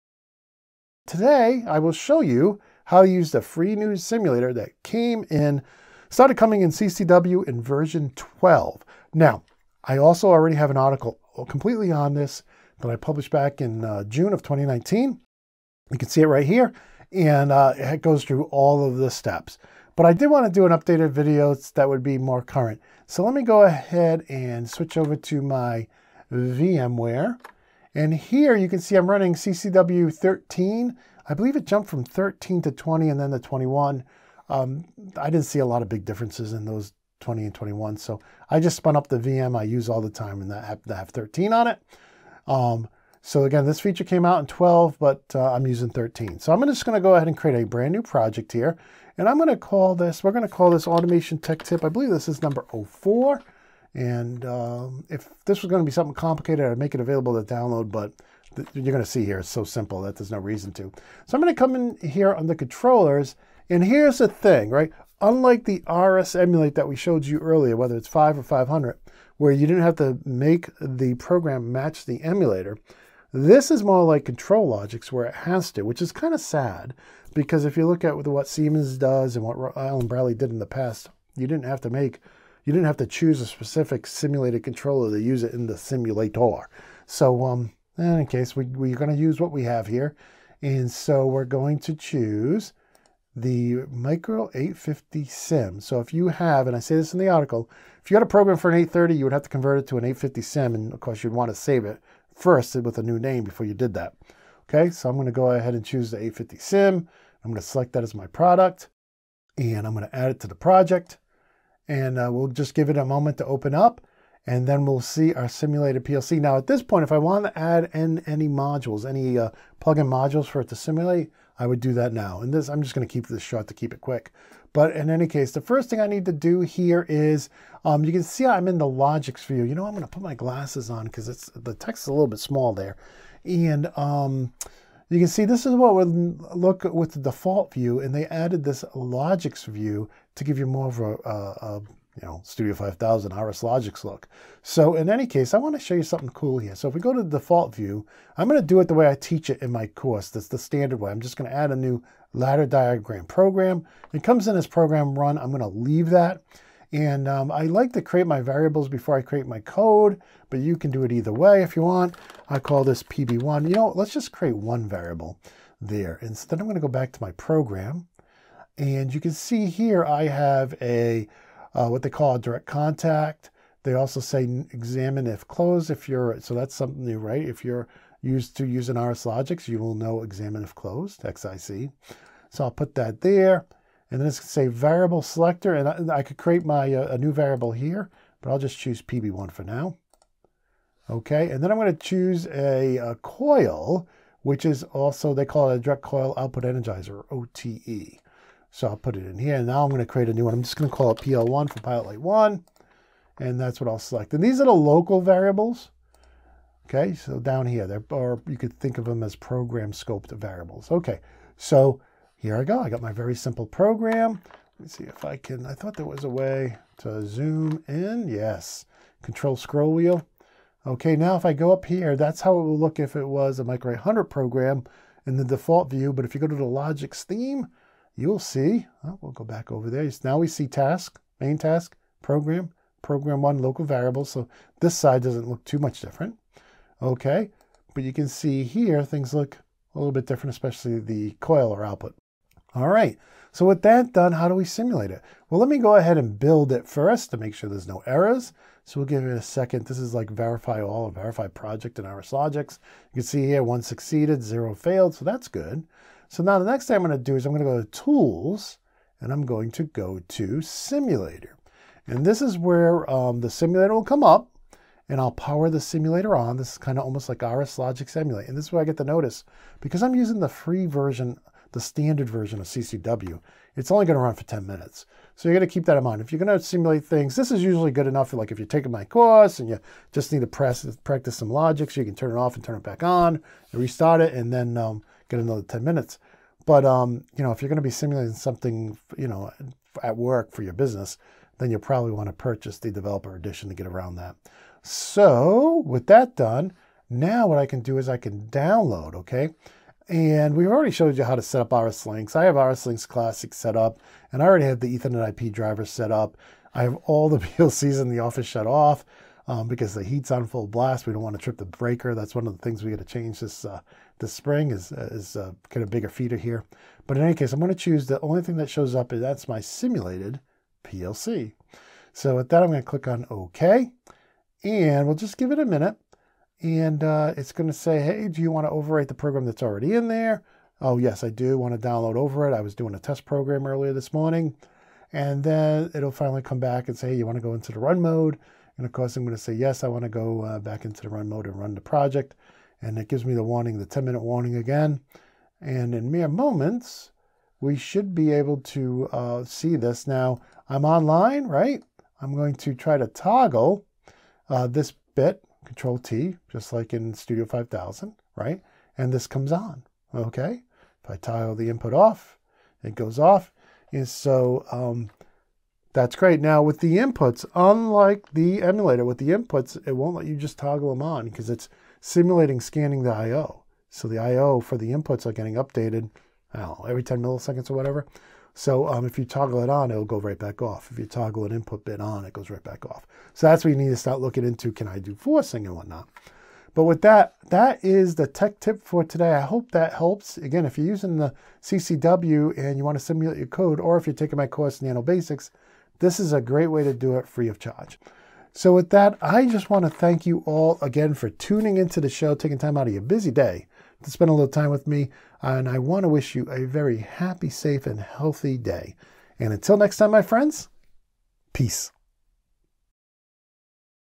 How To Use The Micro800 PLC Simulator in CCW
Shawn shows how to use the free PLC Simulator for the Micro800 that comes with CCW in episode 4 of Automation Tech Tips.